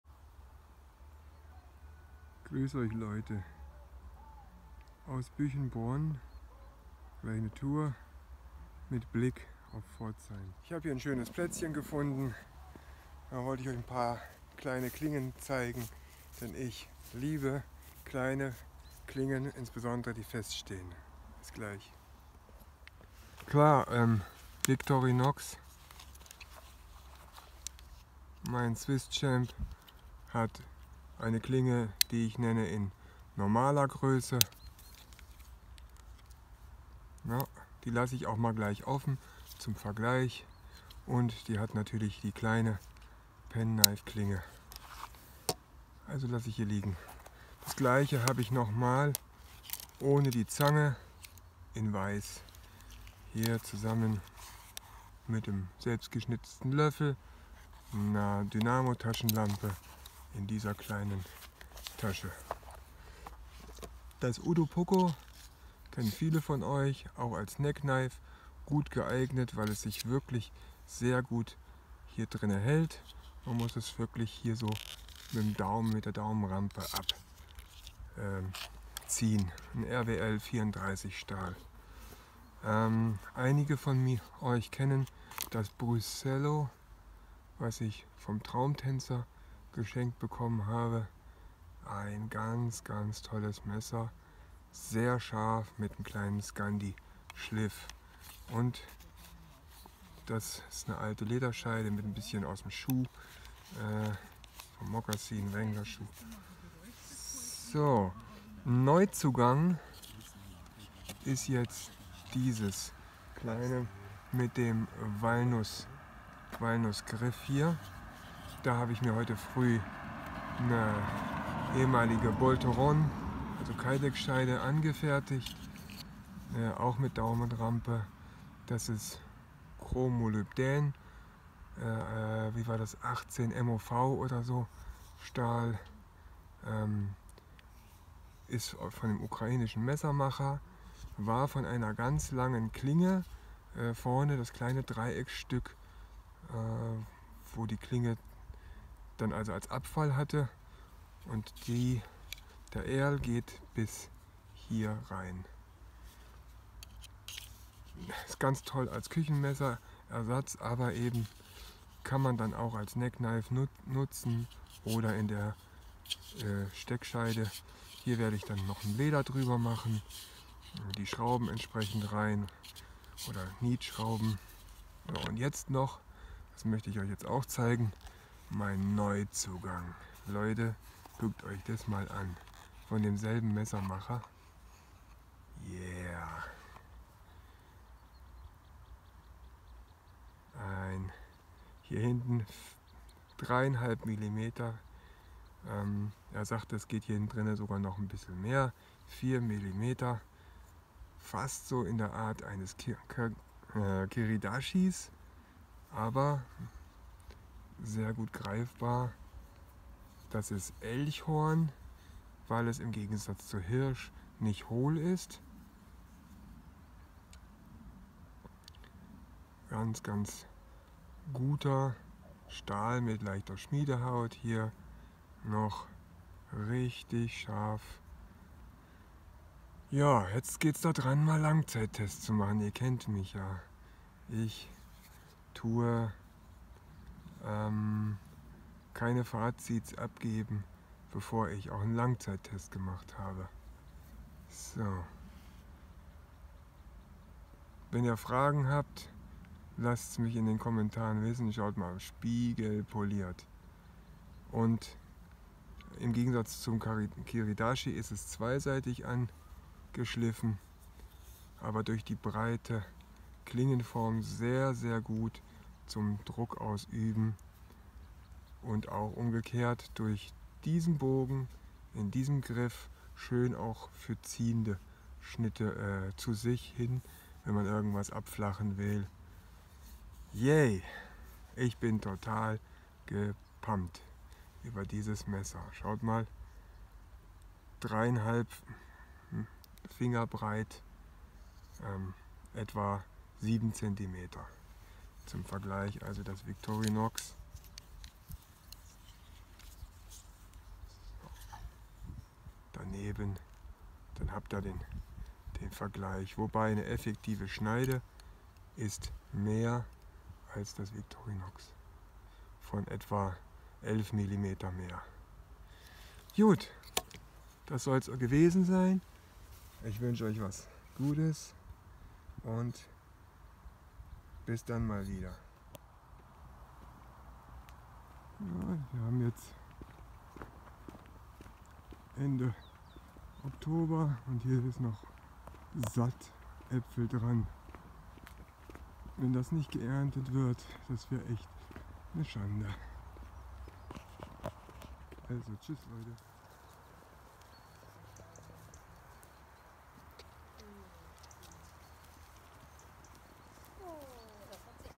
Ich mag kleine (feststehende) Klingen
Ich mag kleine (feststehende) Klingen. Praktisch, zumeist stabil, Paragraf 42a konform, usw.
Bei einem Spaziergang südlich von Büchenbronn habe ich ein paar feststehende Taschenmesser ausgebreitet. Ich zeige meine Kleinsten Klingen und meinen Neuzugang.
Eins davon bekommt jetzt eine innen liegende Kydex Messerscheide mit Lederüberzug.
Das letzte gezeigte Messer ist mit seiner Optik und der Spiegelpolitur für mich etwas Besonderes und Anderes. Dabei haben mich auch die verwendeten Materialien angesprochen.